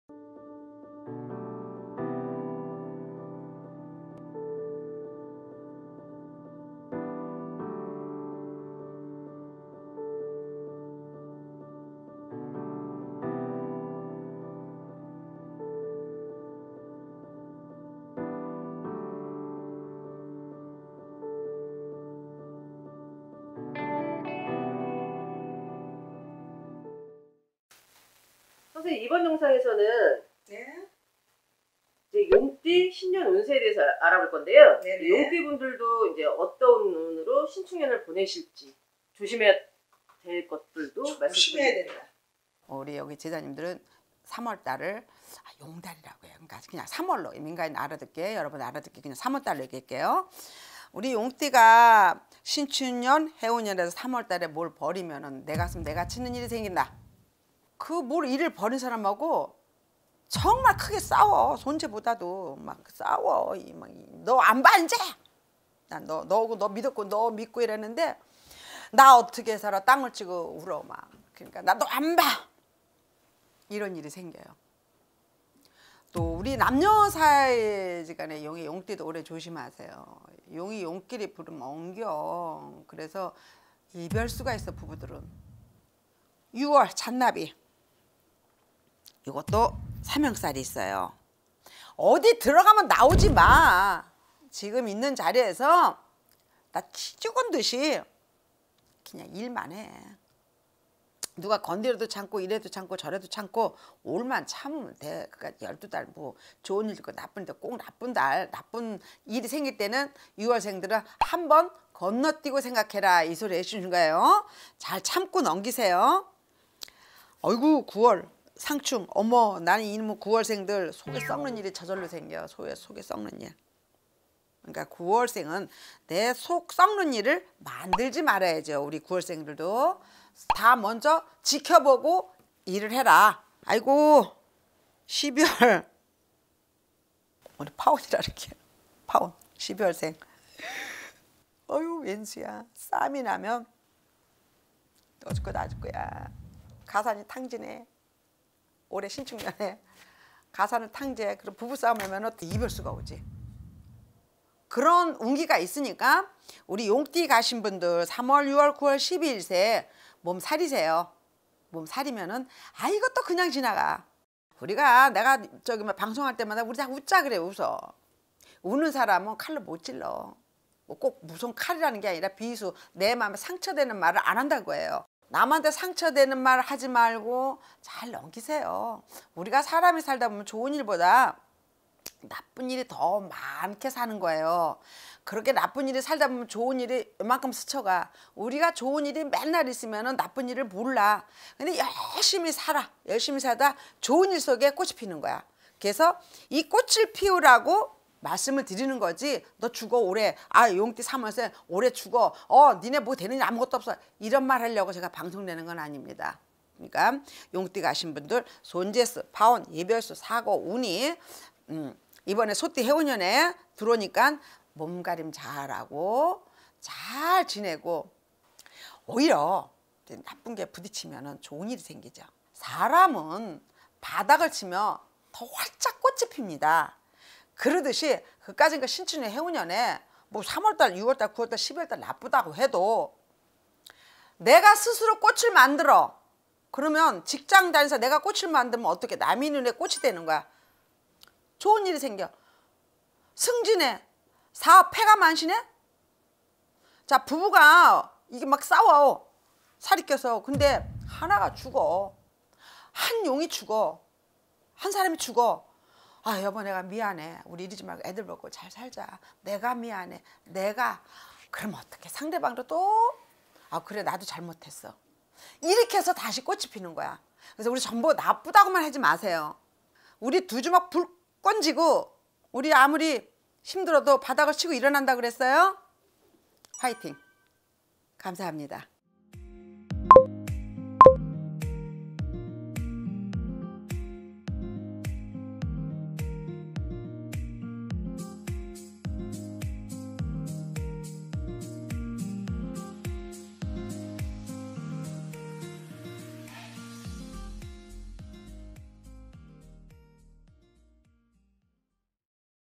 ★ 2021년 용띠운세 ※ 상충이드는띠 / 피해야할달을 알려드립니다.조심히 넘기시면 웃을일만 있어요! ( 서울점집 서울용한점집 서울소문난점집 서울유명한점집 ) 경복궁 대궐할머니
자막 : 나눔템플릿 👉자형살 // 자신이 스스로가 일을 저지르며 부모 육친간에 이별수 파운이 드는살이다

👉삼형살 // 사고 관재 구설 낙상수 같은 일들이 드는살이다

👉청파살+상충살
 // 어떠한 일이든  성사가 되지 않고 일이 깨지고 부딛치게 만든다 ( 주변사람들과 충돌을 생기게 하는 살이다 )

👉원진살 // 서로 망하는 살이다 사람끼리 원수지간을 만드는살이기 때문에  합을 이루면 절대 안된다

★대표블로그★
-올마이티TV